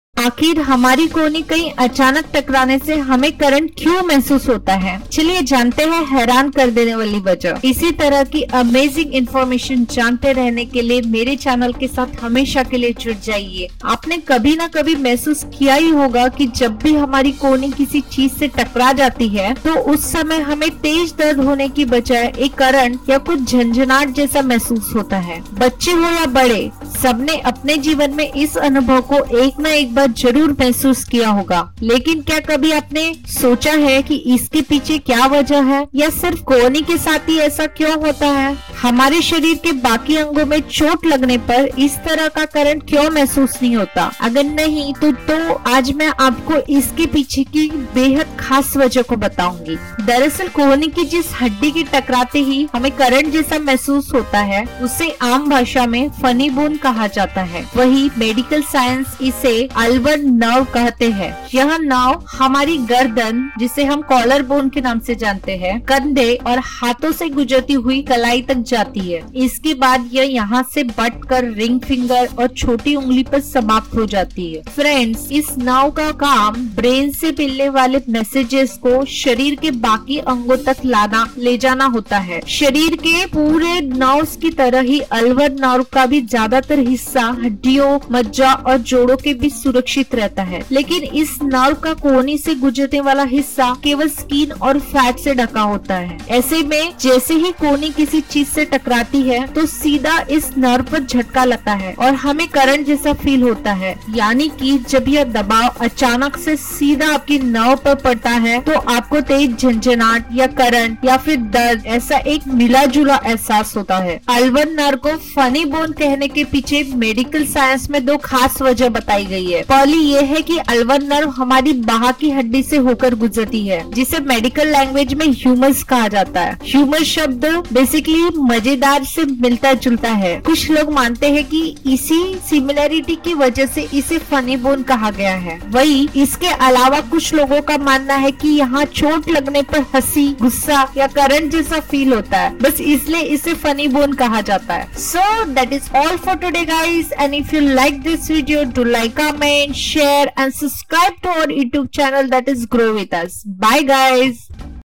Why do we get electric shock on elbow | Aakhir kyun lagta hai current kohni takrane se "Explained"
Lets Learn Something New this Navratri

The ulnar nerve is a long nerve that extends from the neck to the pinkie finger. It is unprotected in the cubital tunnel, which is the space between the upper arm and forearm. When you hit your funny bone, the ulnar nerve is compressed and sends waves of pain through your arm.

As you reach back to pull your chair and sit up straight, your elbow smashes into the chair’s arm, hitting directly on the “sweet spot.” Waves of pain, what people often describe as an “electric shock,” promptly ripple through your arm, from the mighty elbow to your little pinkie.

But what’s so special at this point? Why does it hurt so terribly?

Funny Bone
The spot is called the funny bone, although it is anything but a bone and certainly does not make you laugh. Whether the spot is called “funny” because of the uncanny sensation it evokes or is just a pun on the upper arm, the name of the bone under which this area is located, is still unclear.

The funny “bone” is actually a long vine of a nerve called the ulnar nerve that emanates from your neck, runs alongside the shoulder, bicep, and forearm, enters the wrists, and finally culminates halfway to the ring finger and the tip of your pinkie. The nerve is named after the long bone called the “ulnar” that constitutes your forearm and whose sharp, protruding edge forms your elbow. The bone locked with the ulnar is the humerus, which is the bone above your elbow.

Every nerve in our body is covered and protected by the muscles it innervates. The same applies to the ulnar nerve: it is protected by the flesh and muscles that make up the biceps, forearm, ring finger, and pinkie. However, there is a gap between the upper arm and forearm, the so-called cubital tunnel, where the nerve is most vulnerable: the skin and some fat only protect it as it passes through the cubital tunnel.

When you hit your funny bone against something, the unprotected nerve is pressed against the bone. The squeezed or irritated ulnar nerve spouts the waves of pain, emitting the “electric shock.” The waves terrorize the regions innervated by the nerve: the forearm, the pinkie, and half of the ring finger.